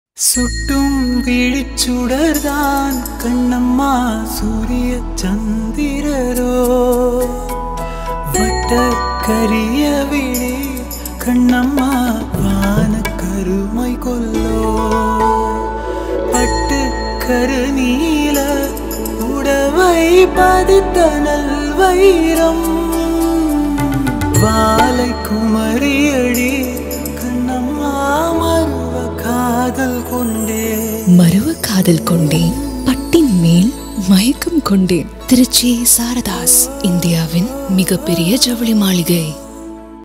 Trichy Sarathas Textiles Silk Saree Tv Commercial | Actress Priyanka Kumar | Santhosh Photography
We are honoured to have produced a TVC , inspired by the words of Bharathiar, for India's Largest Silk sarees showroom, with an 85 year legacy. Trichy Sarathas.